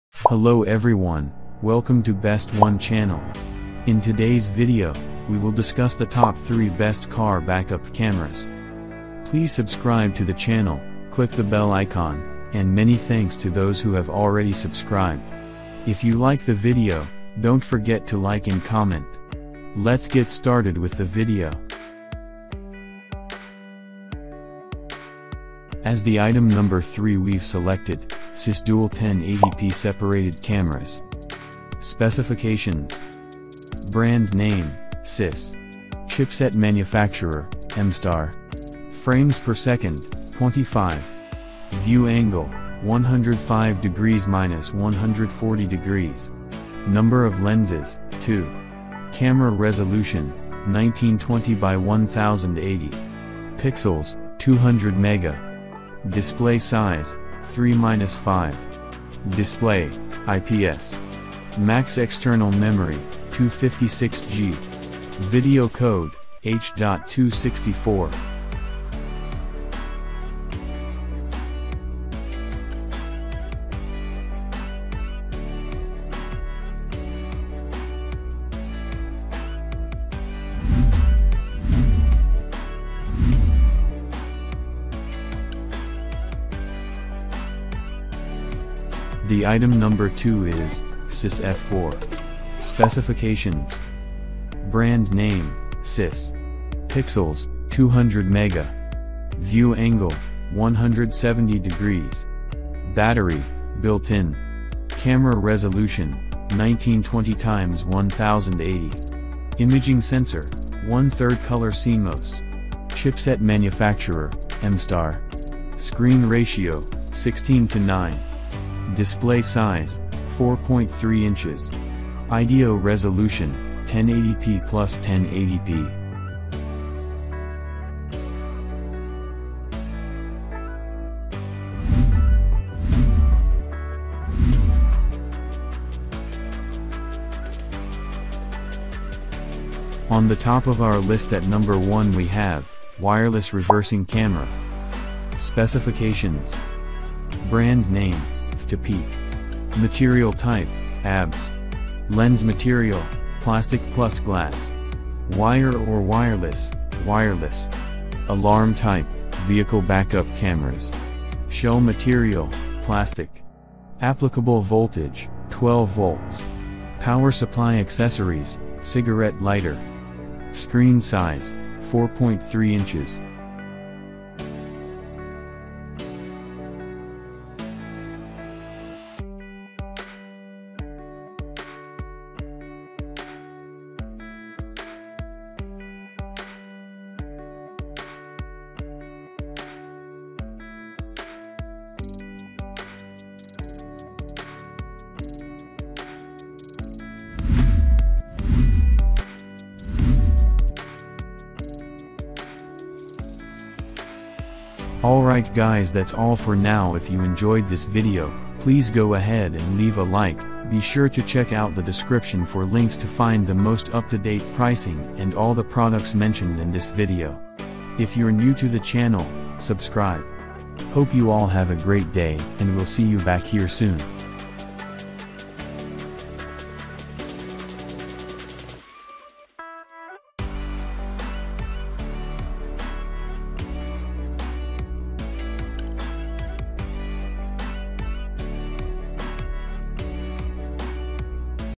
Top 3 Car Backup Cameras in 2022 | aliexpress
Top 3 Car Backup Cameras in 2022
3.VSYS Dash Cam

2.VSYS F4.5 Motorcycle DVR

1.Wireless Reversing Camera

👉👉Stay with us on YouTube👈👈